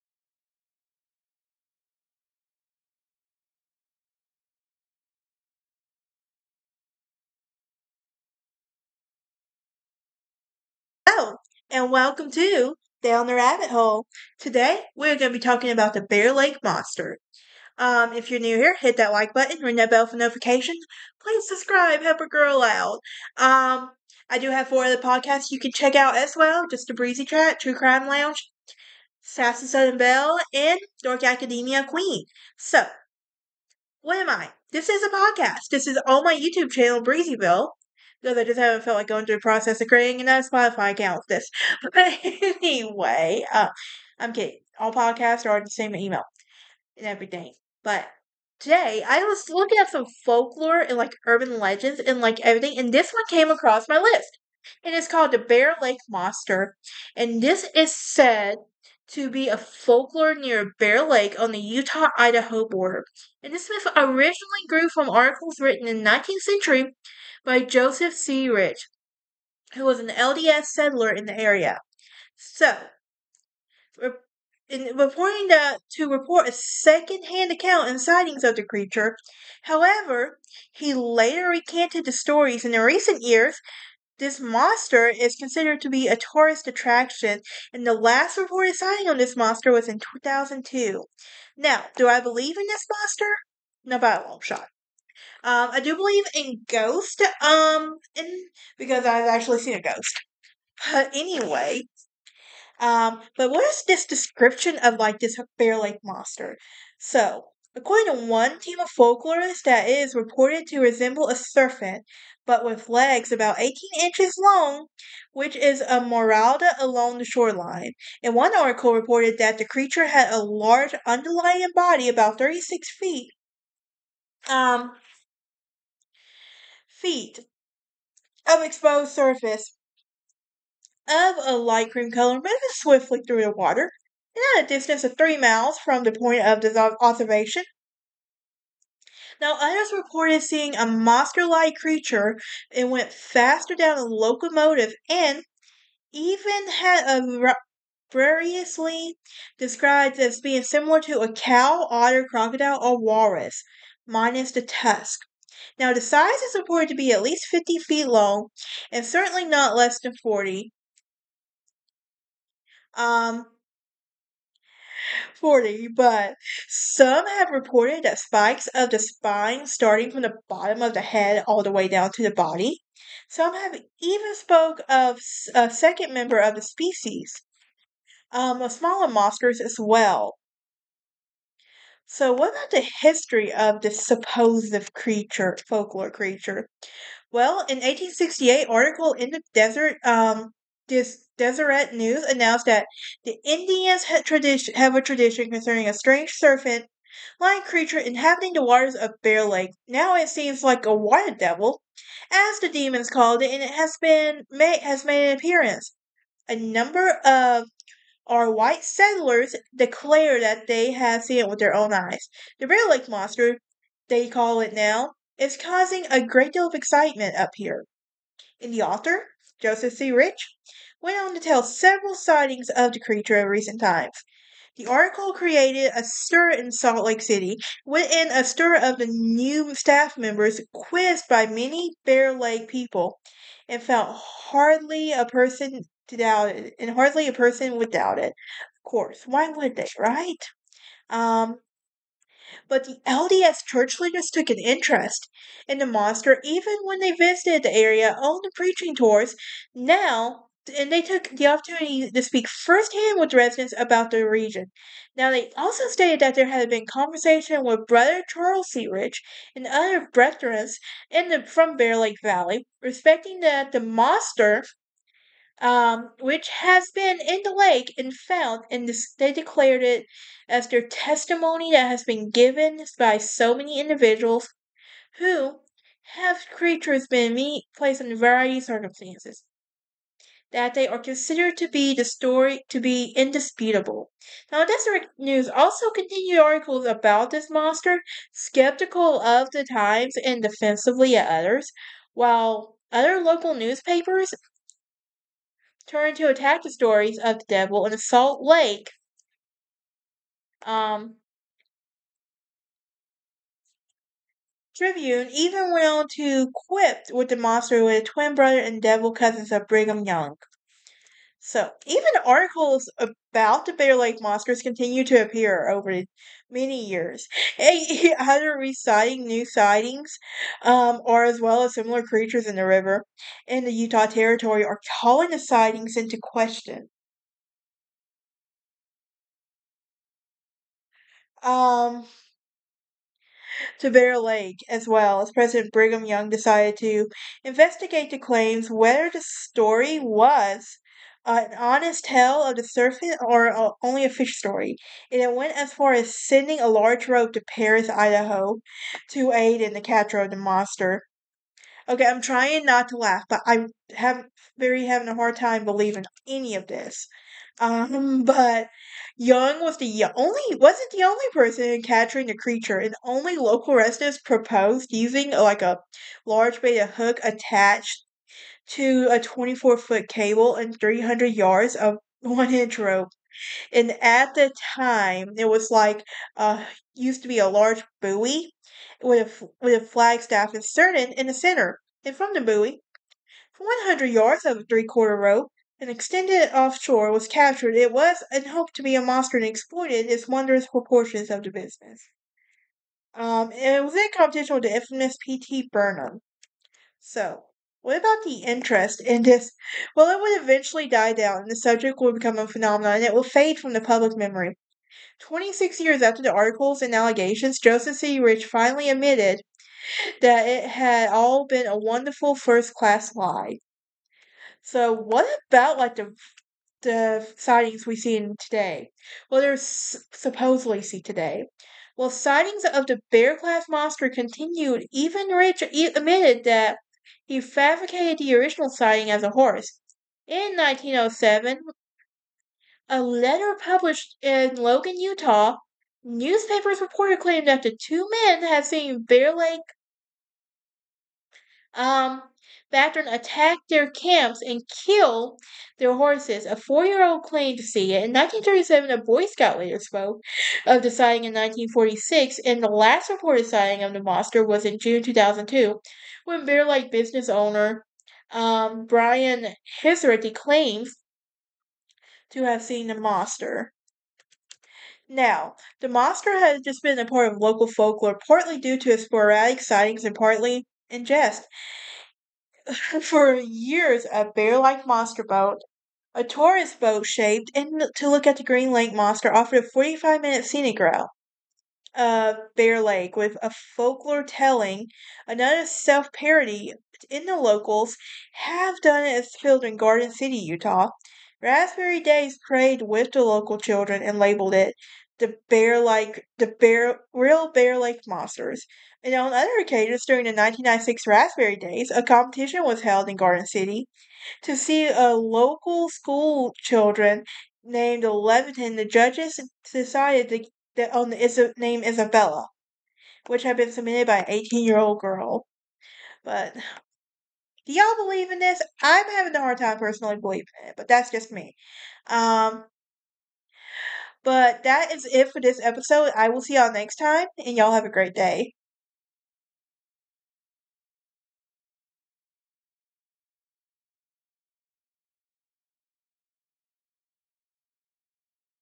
#52: The Legend of Bear Lake Monster
Join me as we talk about his urban legend.